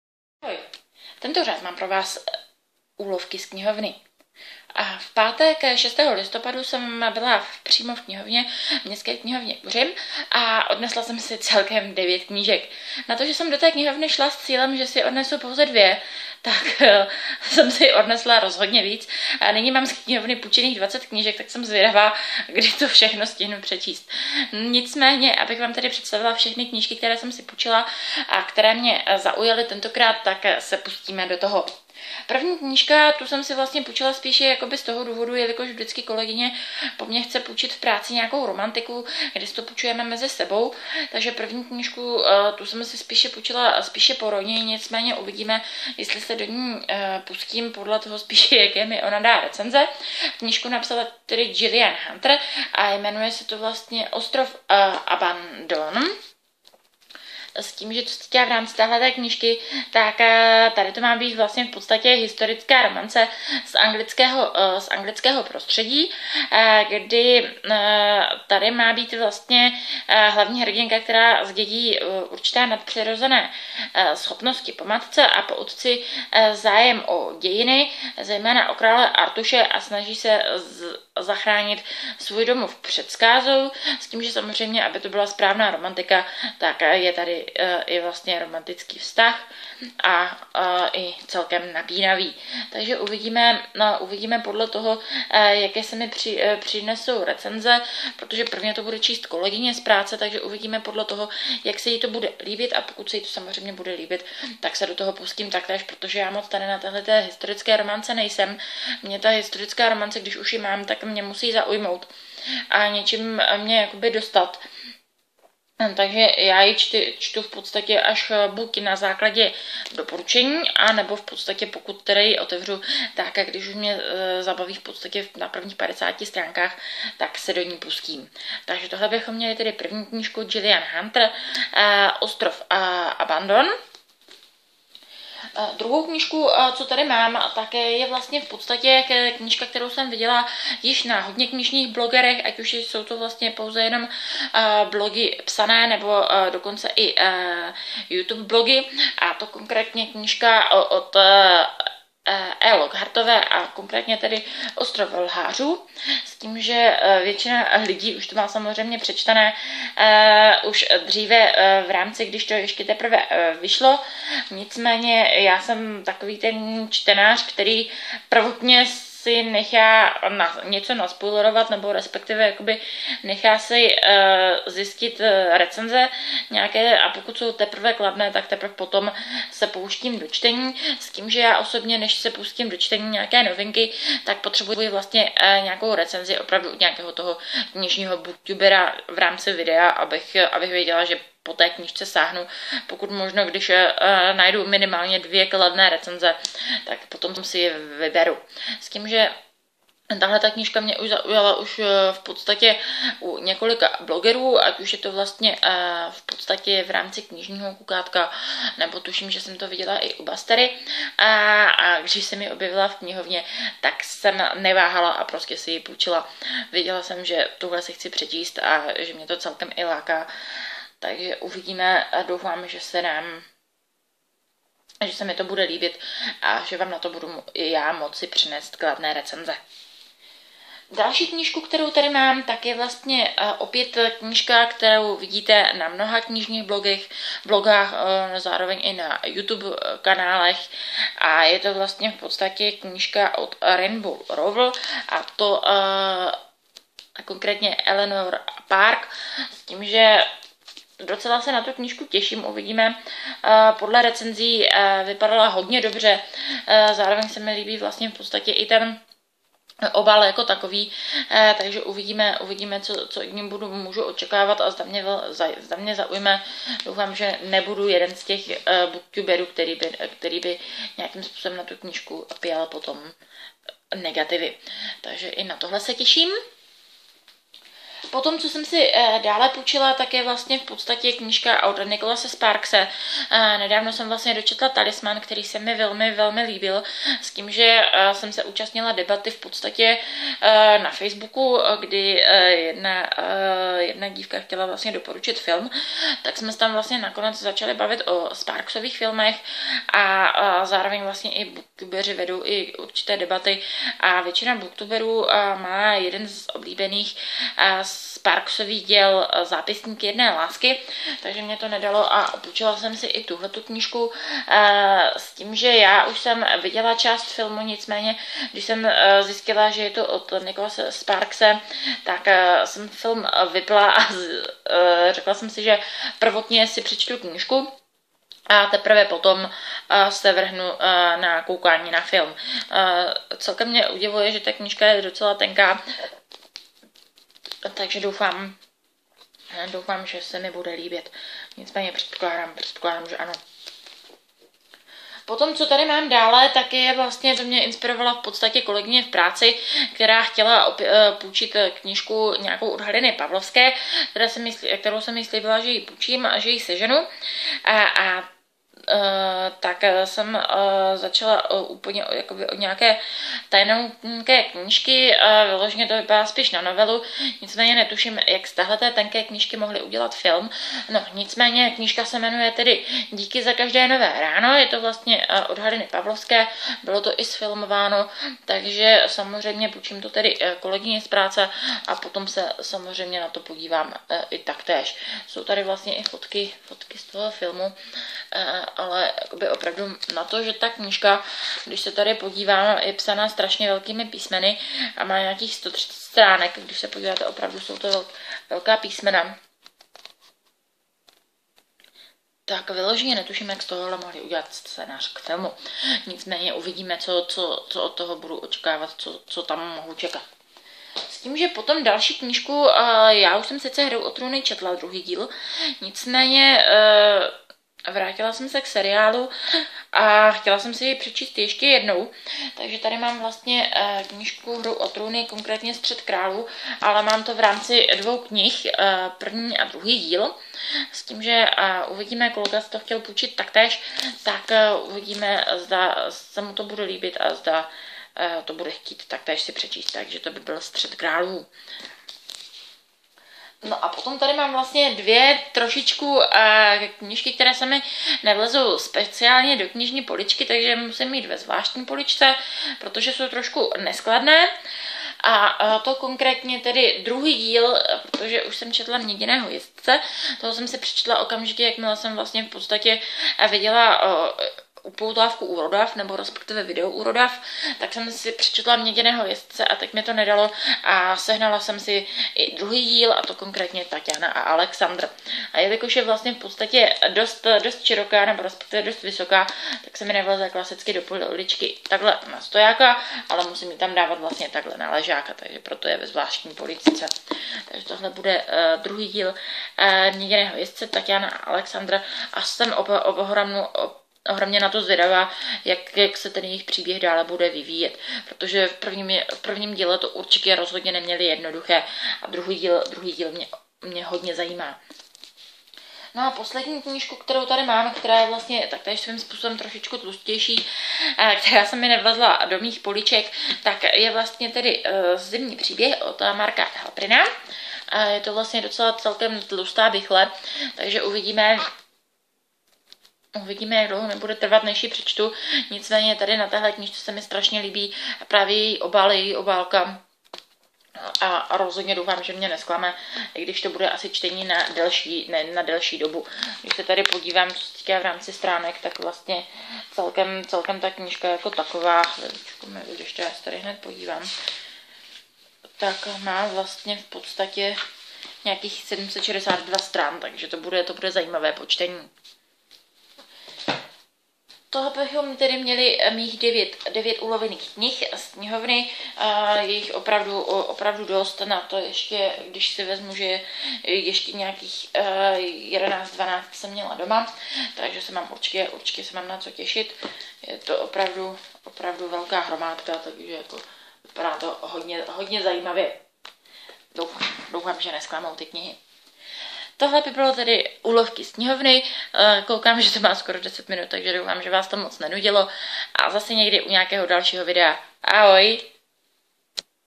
úlovky z knihovny 4
Ahoj přátelé a milovníci knih,
vítám vás u knižních úlovků, které jsem si sehnala v pátek v knihovně. Snad se i vám nějaká kniha bude líbit.
Knihy, o kterých hovořím ve videu:
- Jillian Hunter - Ostrov Abandon,
- E.Lockhartová - Ostrov lhářů,
- Rainbow Rowellová - Eleanor a Park,
- Nicholas Sparks - Zápisník jedné lásky,
- Halina Pawlovská - Díky za každé nové ráno,
- George .R.Martin - Střet králů,
- Paulina Simons - Taťána a Alexandr,
- Mark Helprin - ZImní příběh